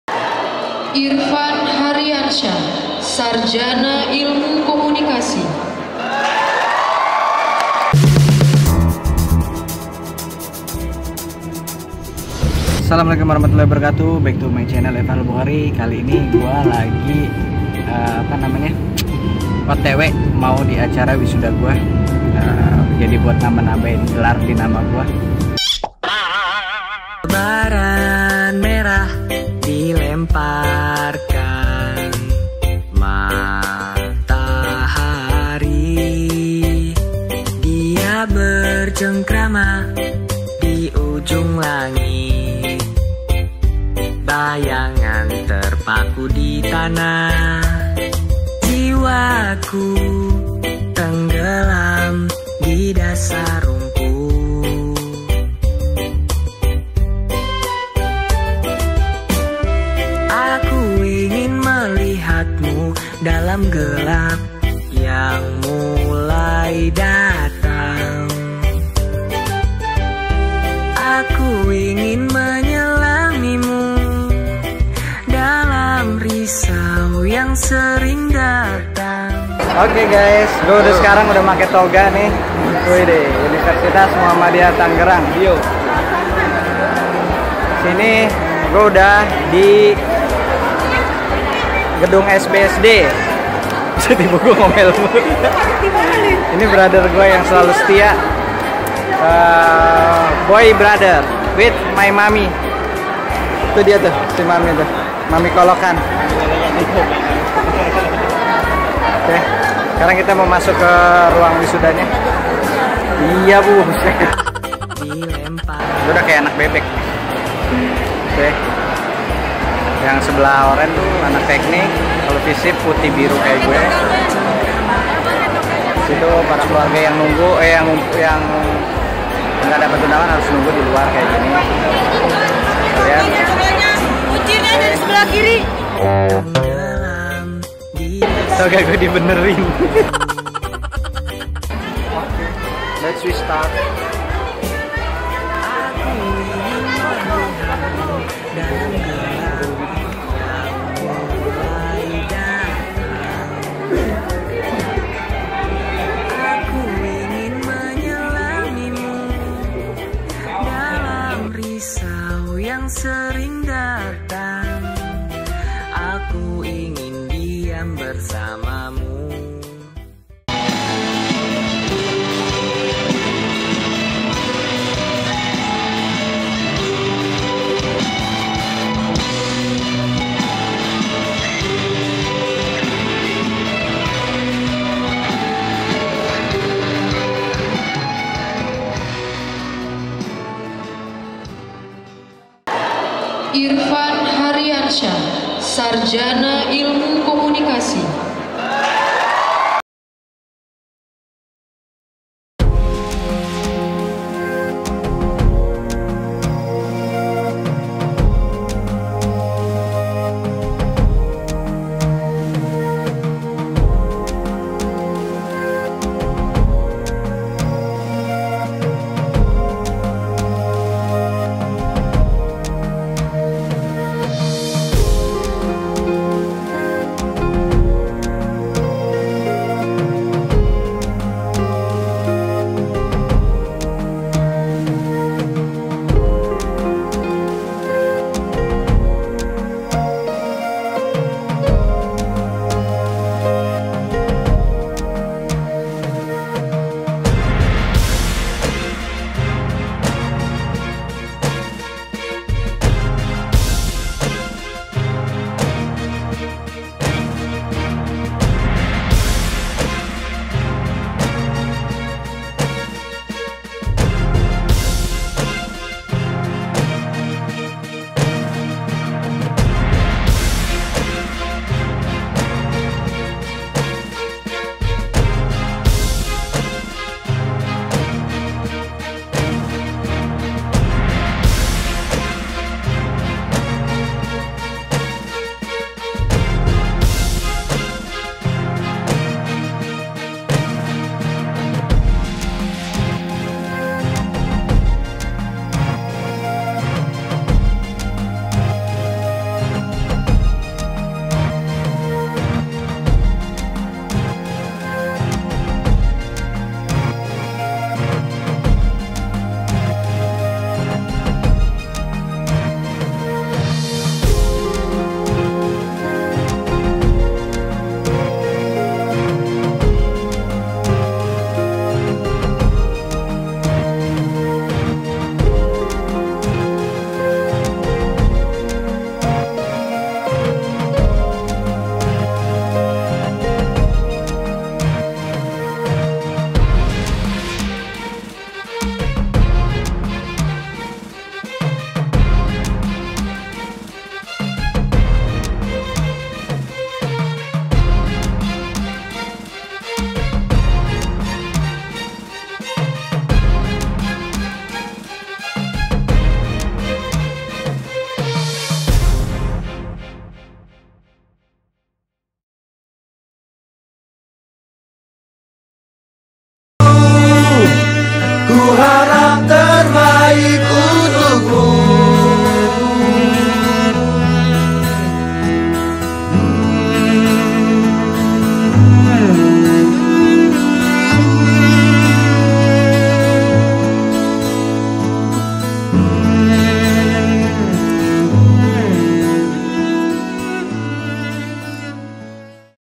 Vlog wisuda - Toga ini milikmu Ibu (No Adsense)
This Video especially for My Mom, (No Adsense) , you can watch for remember your mom anytime.

I just Wanna say : Thank you Allah you gave me one woman best is the world,and she is My Mom.